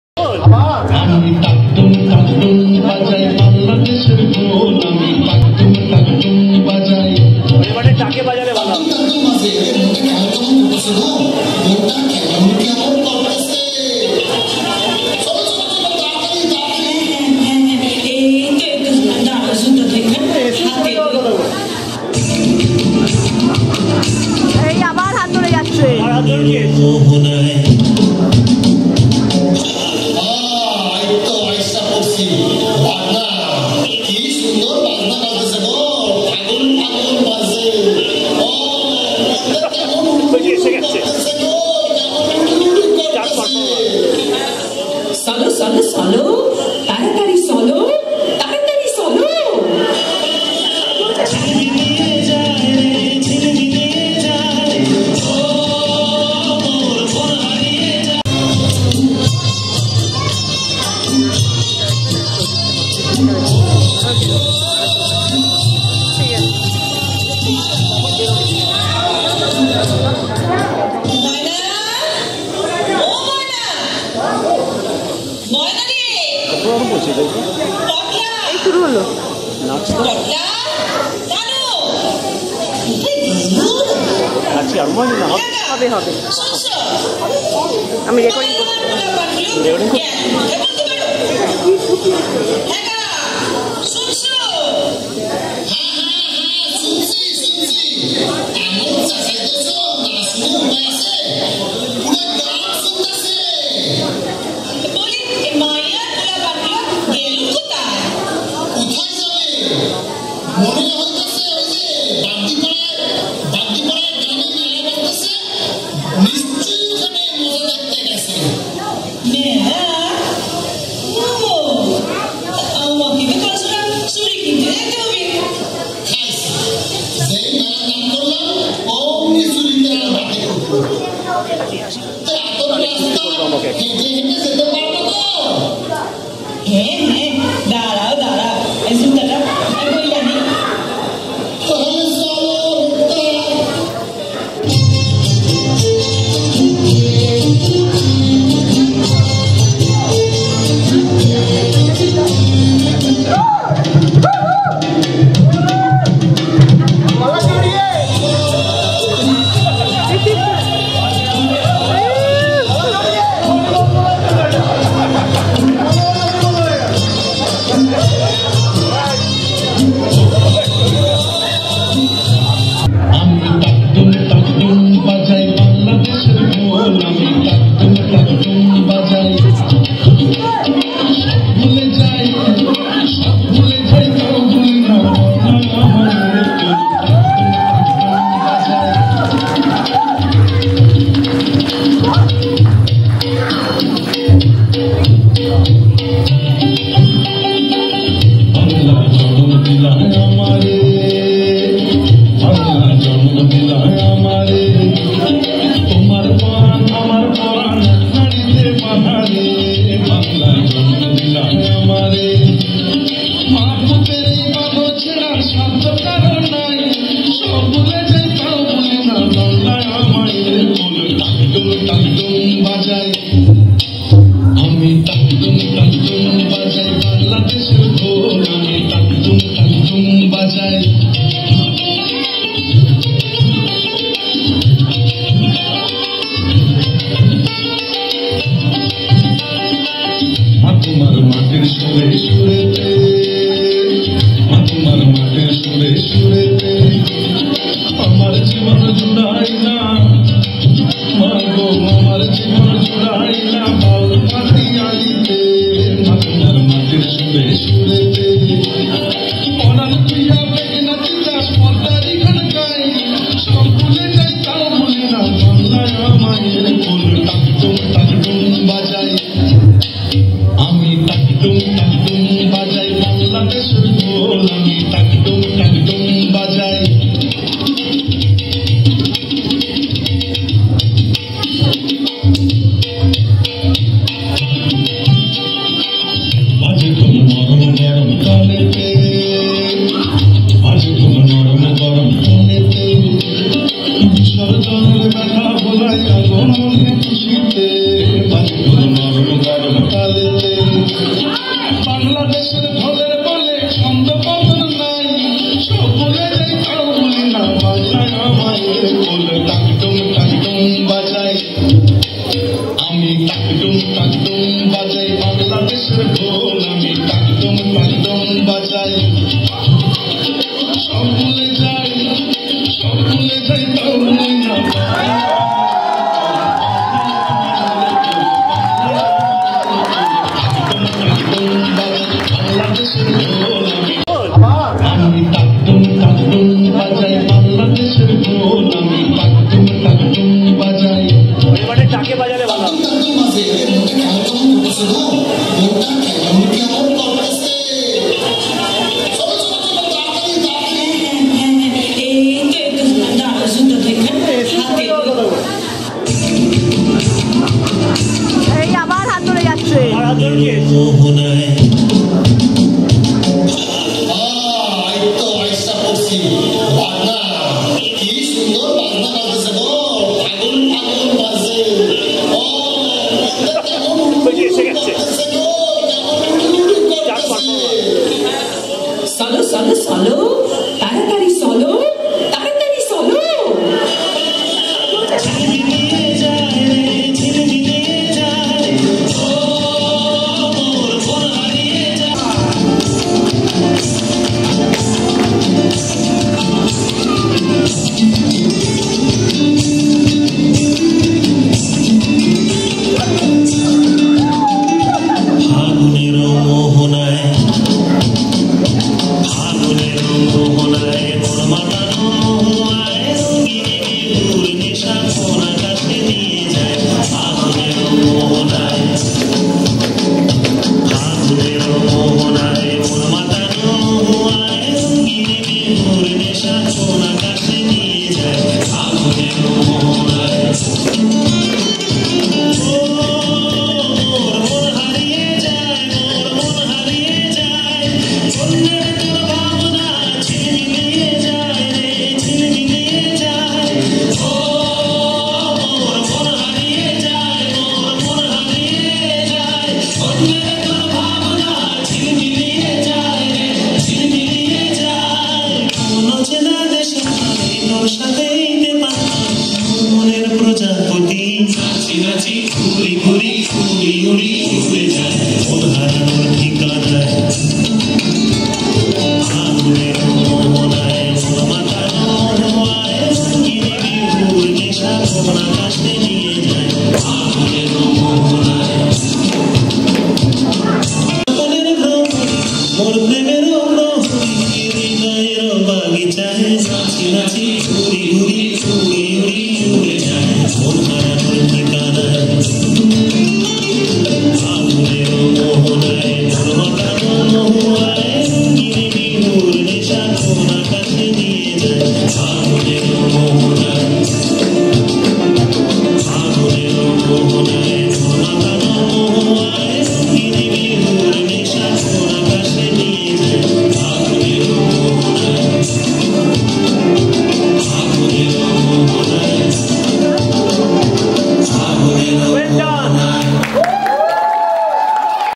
Cultural Program at Tampa Durga Puja '10 - Part 4
Soikot organized Durga Pujo on 16th Oct 2010 at Tampa Hindu Temple.